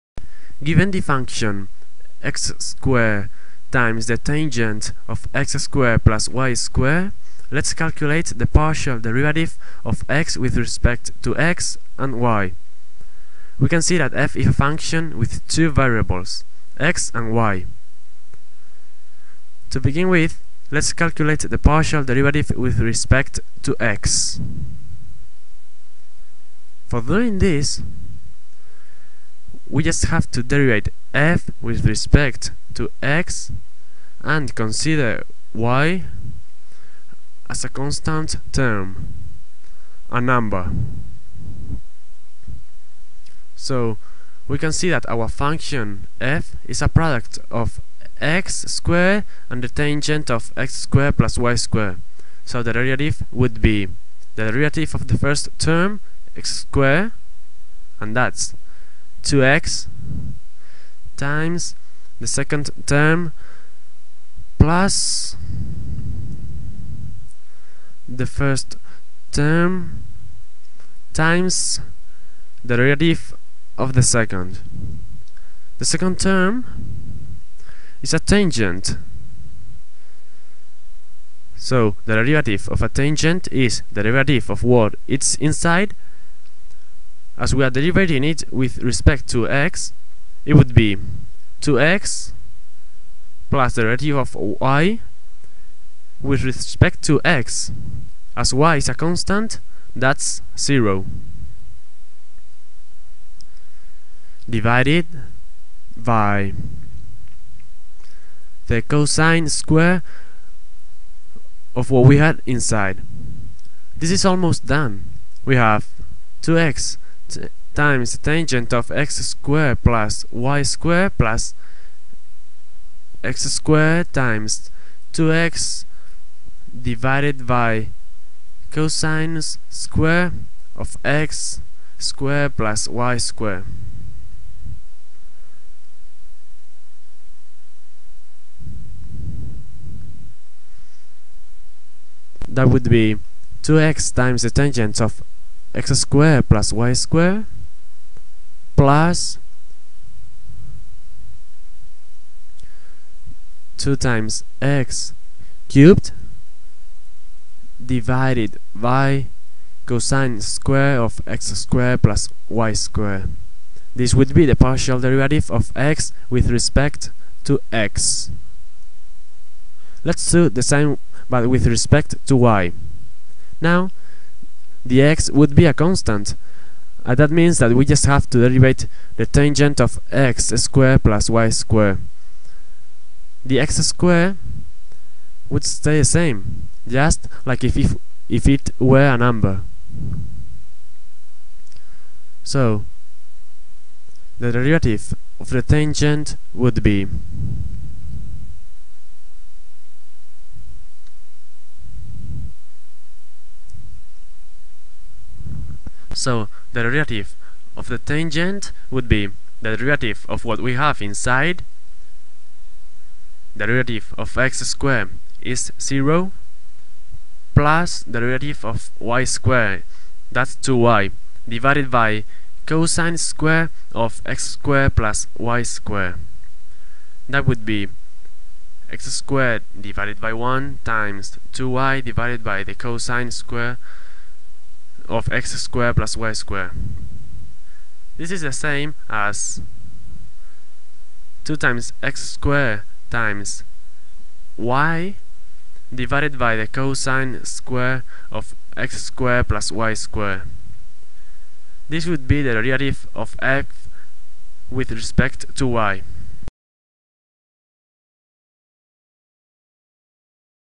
Partial derivatives
We calculate the partial derivatives of a function of several variables.